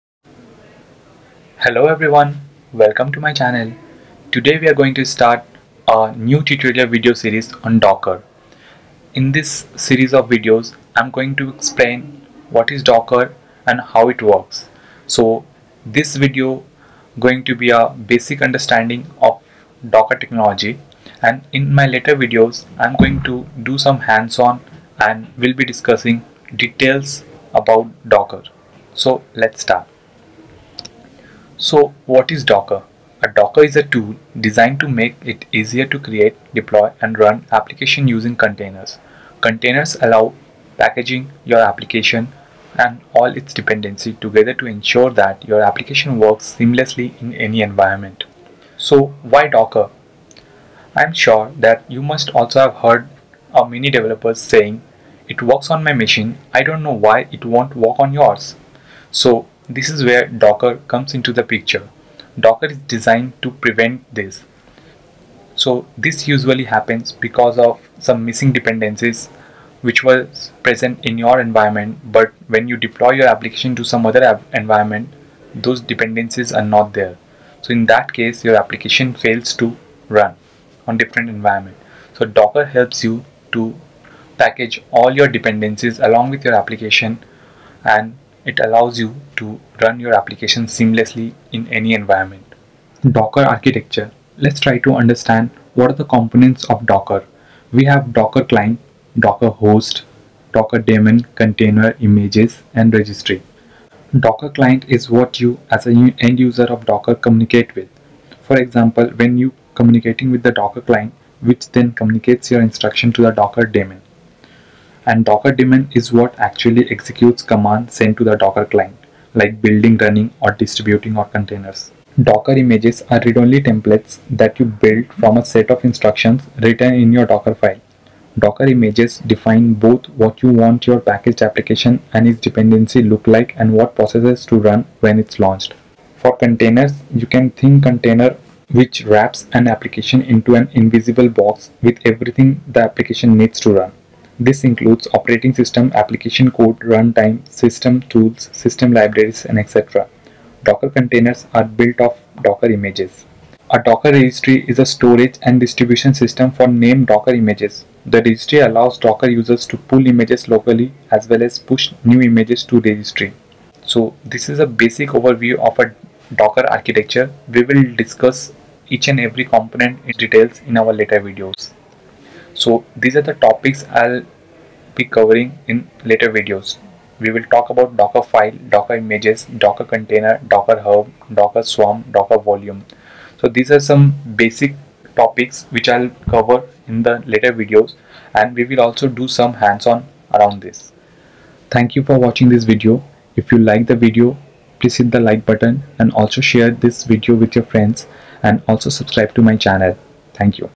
Tutorial #1 | What is Docker? Easy way | Engineer
I have started a new tutorial video series with this video about Docker. Docker is a buzzword and is used almost everywhere. Knowing tools like Docker is a must.

What is docker?

Docker is a tool designed to make it easier to create, deploy and run applications using containers. Containers allow packaging your application and all its dependencies together to ensure that your application works seamlessly in any environment.